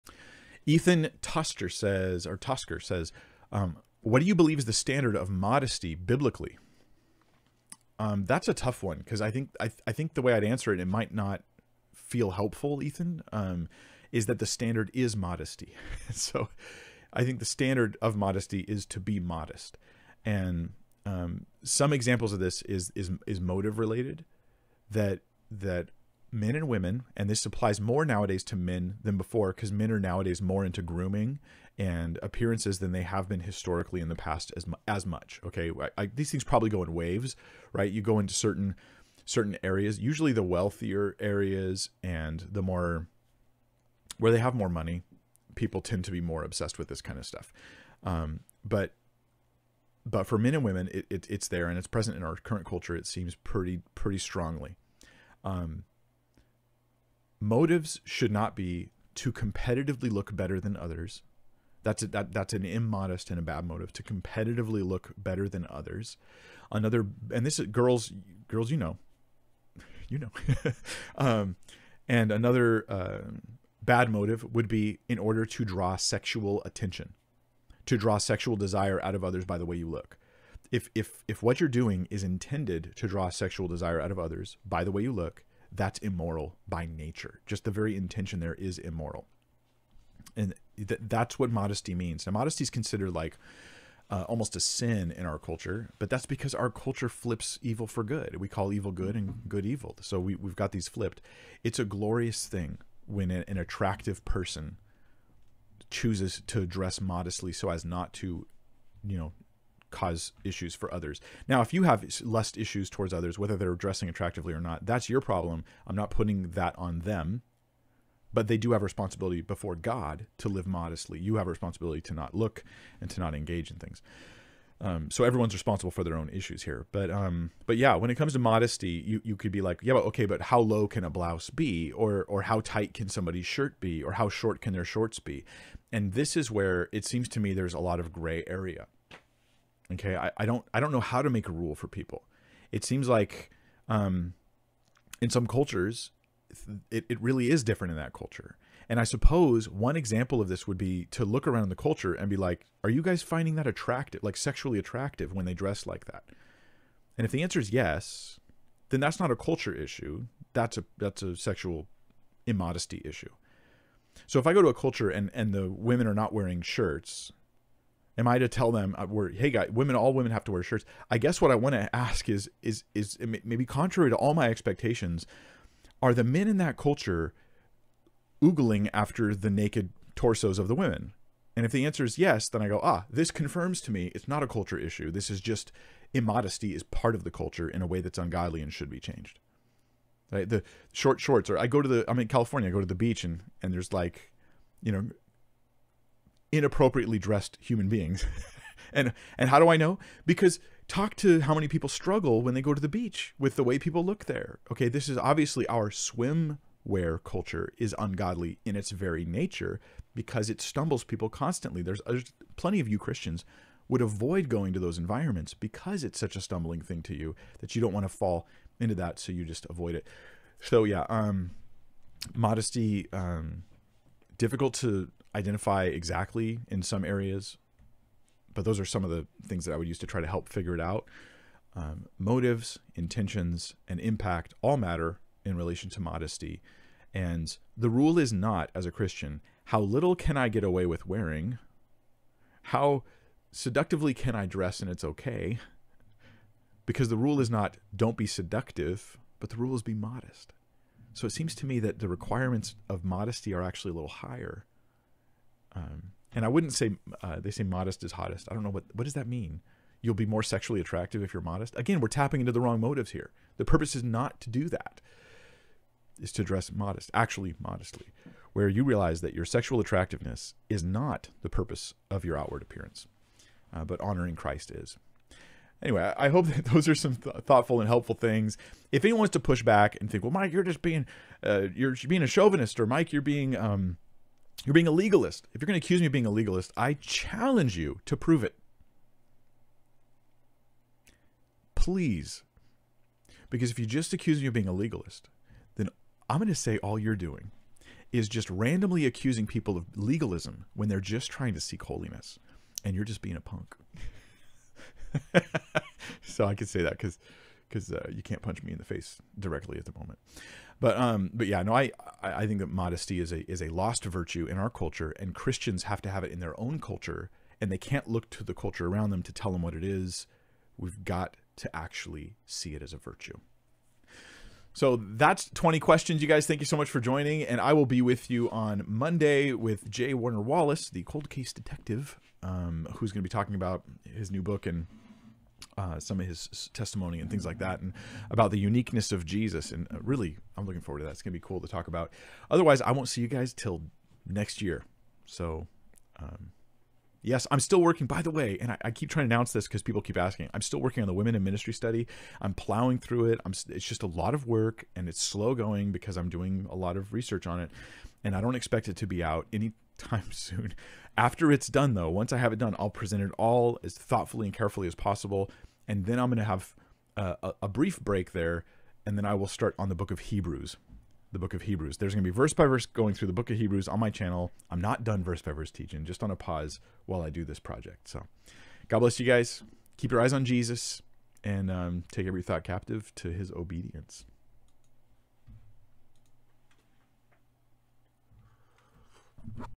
Modesty according to the Bible
{Modesty according to the Bible} What do you believe is the standard of modesty, biblically?